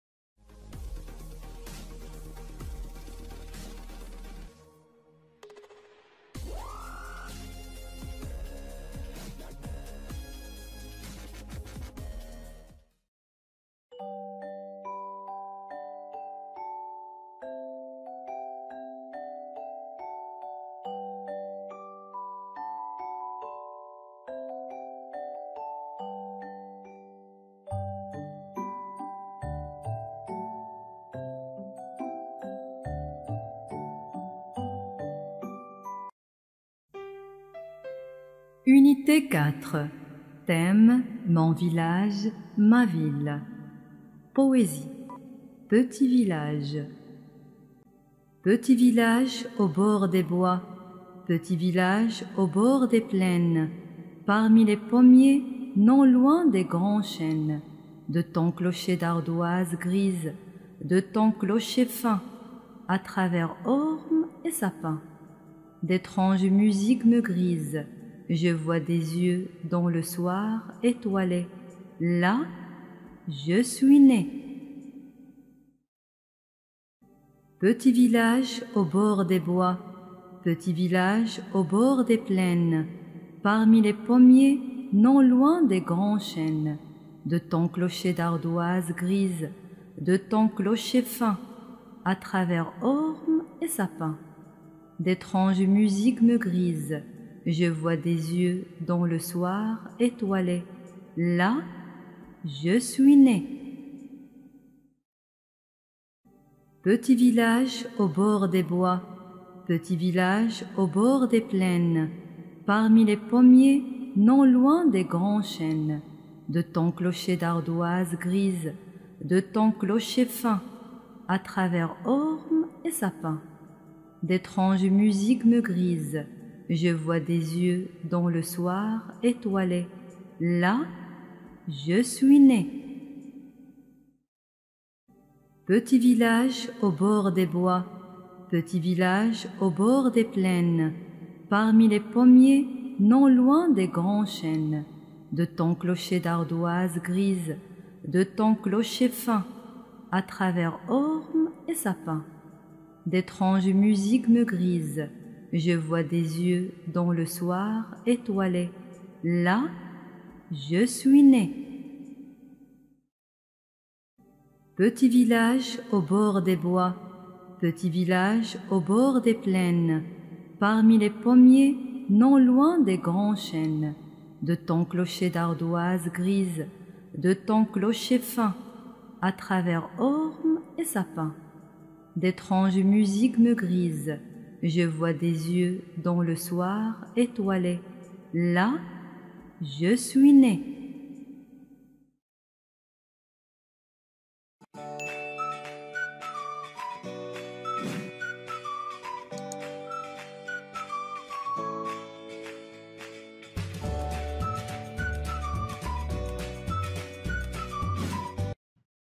poésie : petit village / unité 4 / page 48 / l'école des mots français 2AP
Petit village
Petit village au bord des bois,
Petit village au bord des plaines,
Parmi les pommiers, non loin des grands chênes,
De ton clocher d’ardoises grises,
De ton clocher fin,
A travers ormes et sapins,
D’étranges musiques me grisent ;
Je vois des yeux dans le soir étoilé :
Là, je suis né...

D’après Philéas Lebesgue